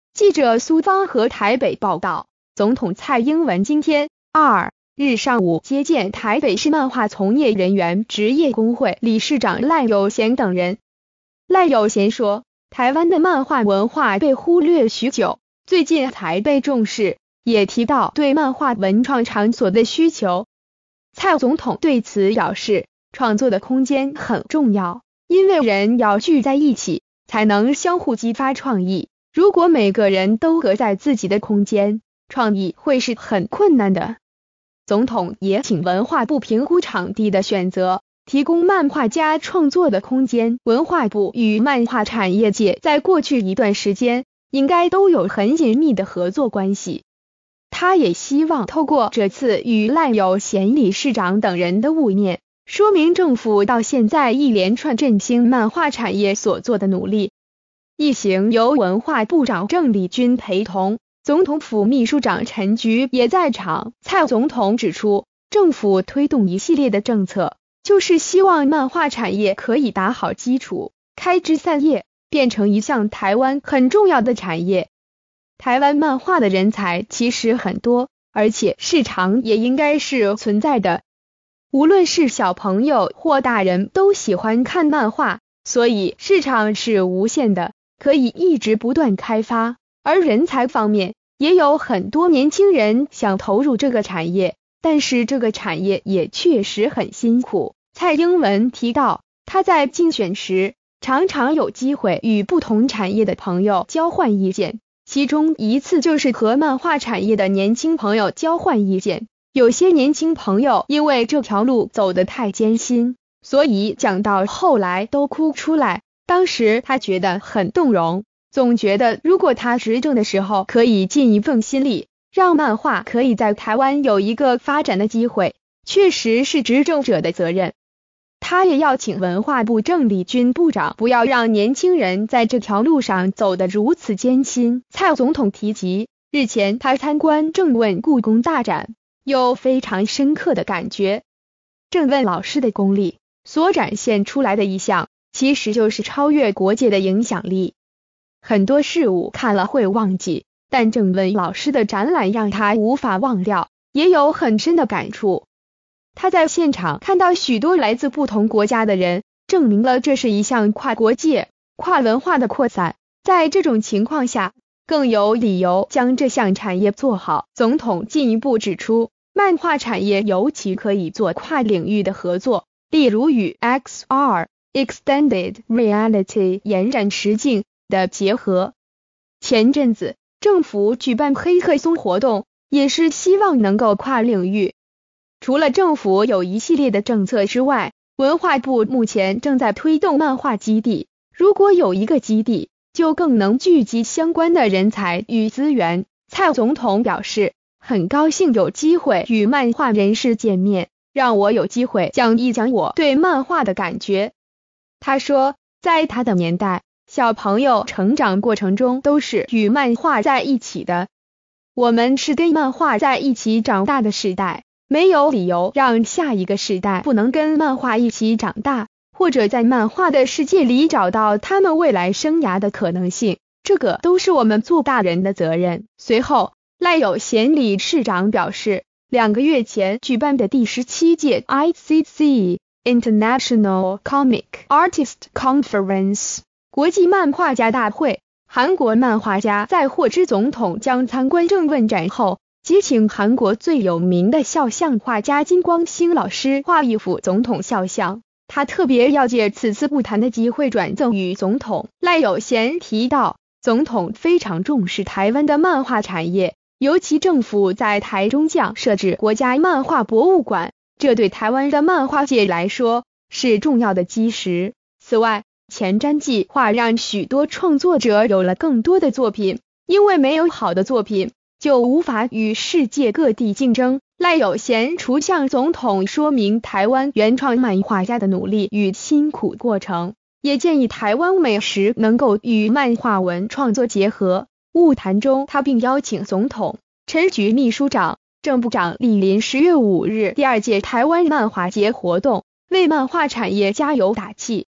提供漫畫文創場所 蔡英文指示文化部評估
〔記者蘇芳禾／台北報導〕總統蔡英文今天（2）日上午接見「台北市漫畫從業人員職業工會」理事長賴有賢等人。賴有賢說，台灣的漫畫文化被忽略許久，最近才被重視，也提到對漫畫文創場所的需求。蔡總統對此表示，創作的空間很重要，因為人要聚在一起，才能相互激發創意，如果每個人都隔在自己的空間，創意會是很困難的。總統也請文化部評估場地的選擇，提供漫畫家創作的空間。

發表評論評論如下。